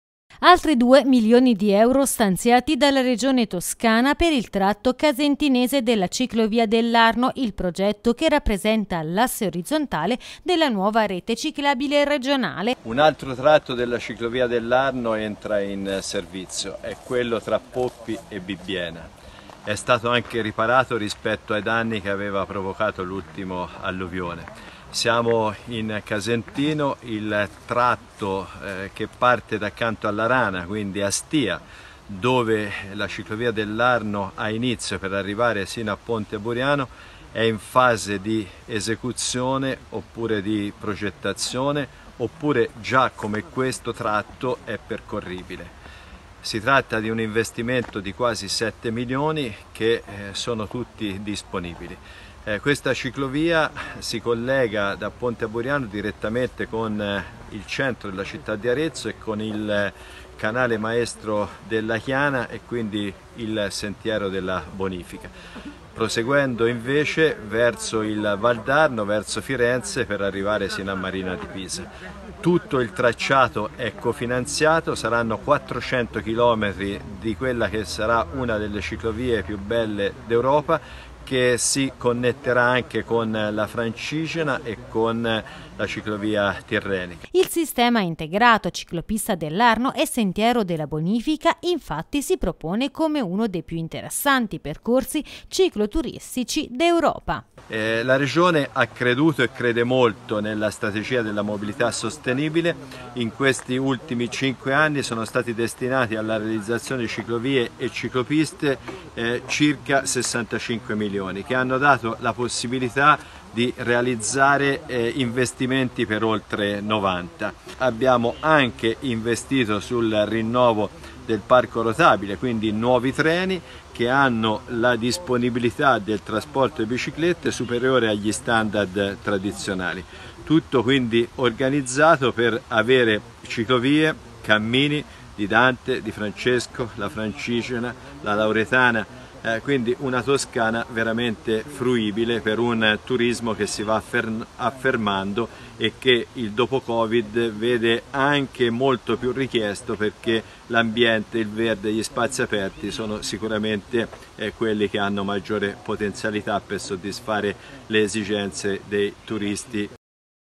Ciclovia dell'Arno, altri 2 milioni di euro dalla Regione per il tratto casentinese
Altri 2 milioni di euro stanziati dalla Regione per il tratto casentinese della Ciclovia dell’Arno, il progetto che rappresenta l'asse orizzontale della nuova rete ciclabile regionale.
Il sistema integrato Ciclopista dell’Arno e sentiero della Bonifica infatti, si propone come uno dei più interessanti percorsi cicloturistici d’Europa.